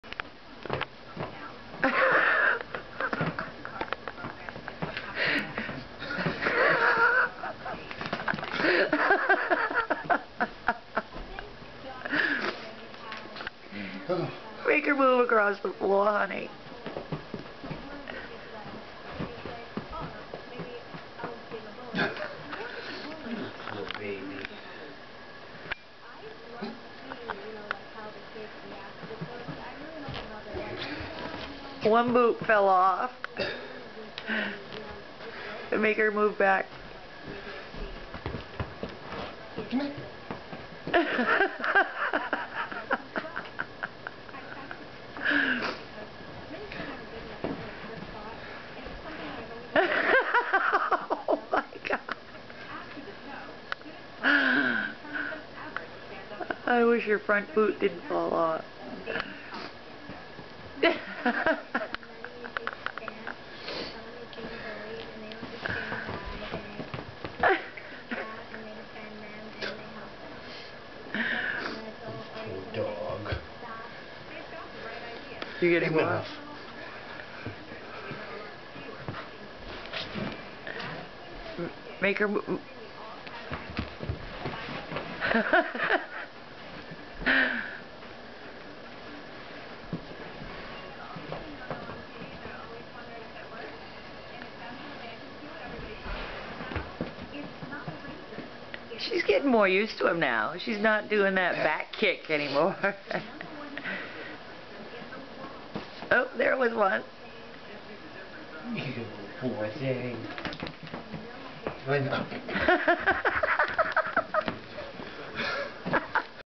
My Dog's New Boots, Izel the Mexican Hairless, Xolo
My 3 year old Mexican Hairless dog, Izel, wearing boots for the first time.